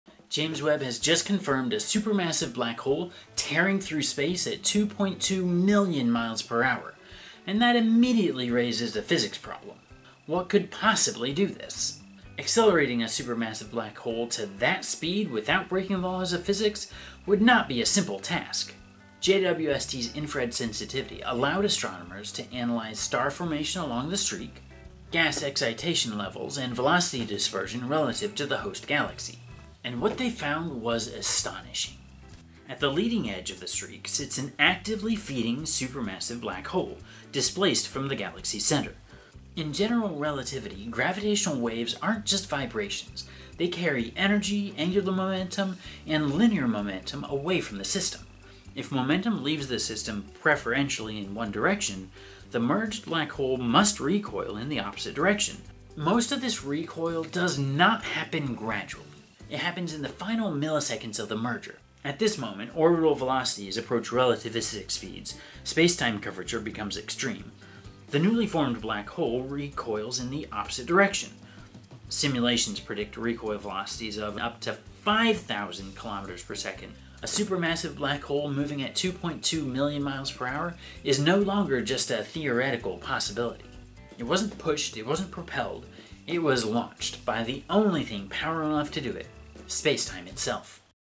Supermassive Black Hole Moving Near Relativistic Speed
JWST James Webb Space Telescope helped confirm the existence of a supermassive black hole moving at 2.2 million miles per hour. We'll be looking at how something that massive could ever reach that speed without violating physics.